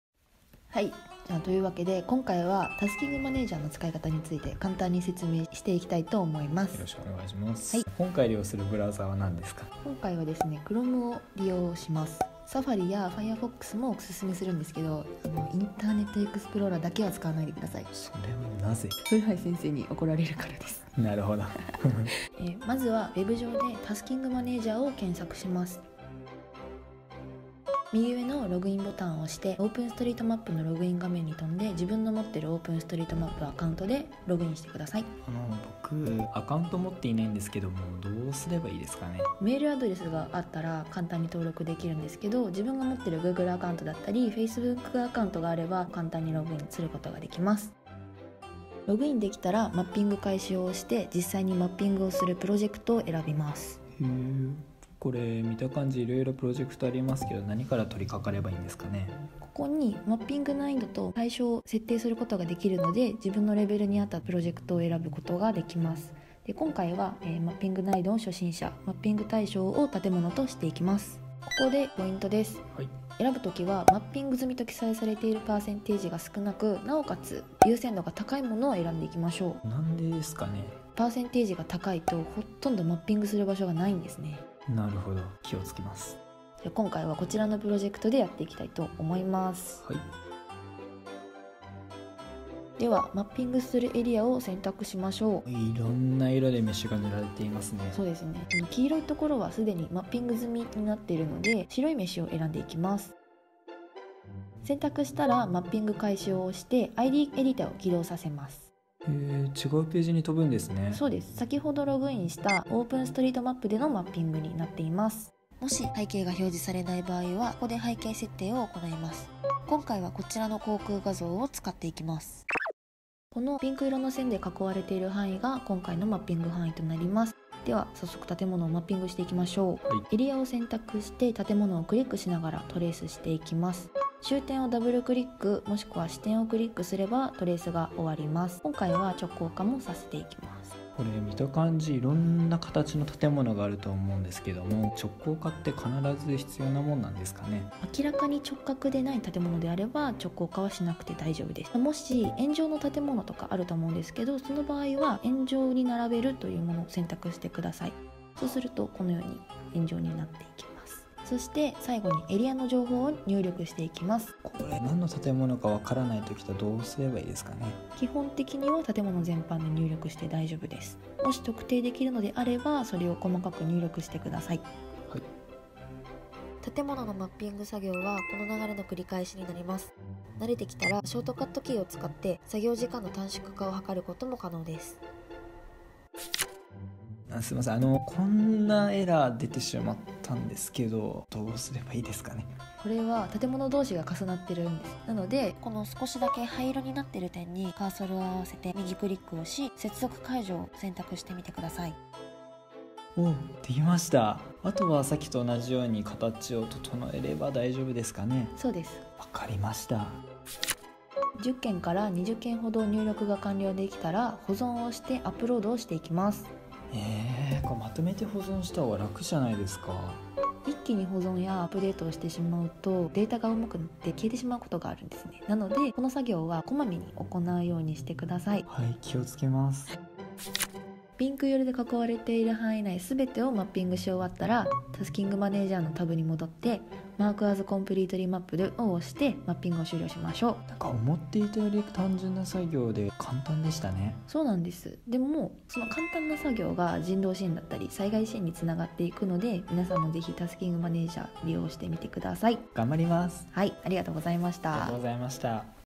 タスキングマネージャーのつかいかた2020 / How to use Tasking Manager 2020 by Furuhashi(mapconcierge) Lab.
OpenStreetMap地図編集ツール iD Editor と作業管理ツールタスキングマネージャの使い方紹介。主に建物入力作業について説明します。

made by Nao Yoshida, Hironori Morita